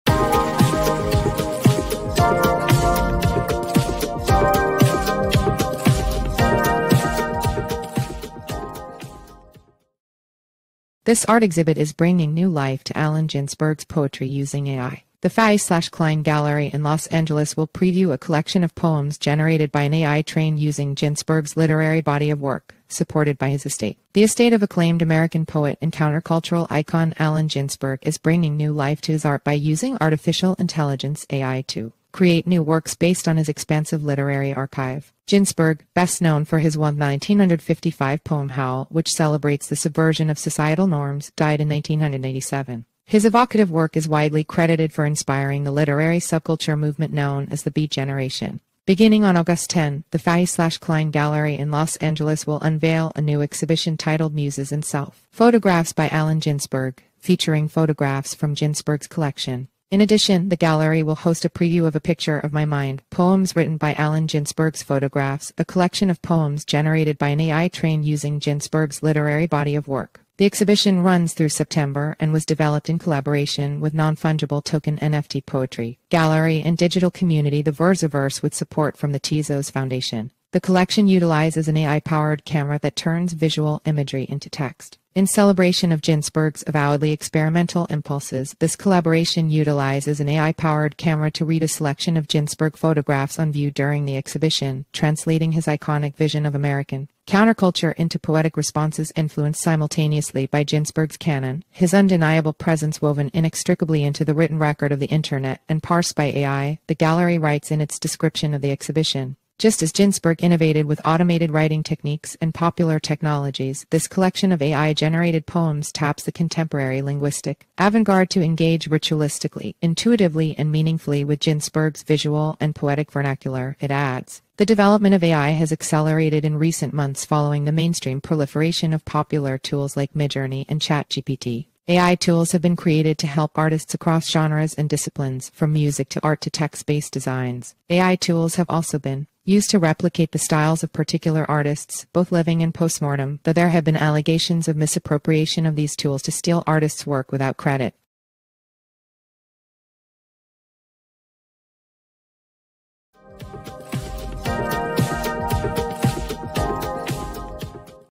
This Art Exhibit Is Bringing New Life to Allen Ginsberg’s Poetry Using AI
The Fahey/Klein Gallery in Los Angeles will preview a collection of poems generated by an AI trained using Ginsberg’s literary body of work, supported by his estate.

The estate of acclaimed American poet and countercultural icon Allen Ginsberg is bringing new life to his art by using artificial intelligence (AI) to create new works based on his expansive literary archive.

Ginsberg, best known for his 1955 poem “Howl,” which celebrates the subversion of societal norms, died in 1997. His evocative work is widely credited for inspiring the literary subculture movement known as the Beat Generation.

Beginning on Aug. 10, the Fahey/Klein Gallery in Los Angeles will unveil a new exhibition titled “Muses & Self: Photographs by Allen Ginsberg,” featuring photographs from Ginsberg’s collection. In addition, the gallery will host a preview of “A Picture of My Mind: Poems Written by Allen Ginsberg’s Photographs,” a collection of poems generated by an AI trained using Ginsberg’s literary body of work.

The exhibition runs through September and was developed in collaboration with non-fungible token (NFT) poetry gallery and digital community TheVERSEverse with support from the Tezos Foundation. The collection utilizes an AI-powered camera that turns visual imagery into text.

“In celebration of Ginsberg’s avowedly experimental impulses, this collaboration utilizes an AI-powered camera to ‘read’ a selection of Ginsberg photographs on view during the exhibition, translating his iconic vision of American counterculture into poetic responses influenced simultaneously by Ginsberg’s canon, his undeniable presence woven inextricably into the written record of the internet, and parsed by AI,” the gallery writes in its description of the exhibition.

“Just as Ginsberg innovated with automated writing techniques and popular technologies, this collection of AI-generated poems taps the contemporary linguistic avant-garde to engage ritualistically, intuitively and meaningfully with Ginsberg’s visual and poetic vernacular,” it adds.

The development of AI has accelerated in recent months following the mainstream proliferation of popular tools like Midjourney and ChatGPT. AI tools have been created to help artists across genres and disciplines, from music to art to text-based designs. AI tools have also been used to replicate the styles of particular artists, both living and post-mortem, though there have been allegations of misappropriation of these tools to steal artists’ work without credit.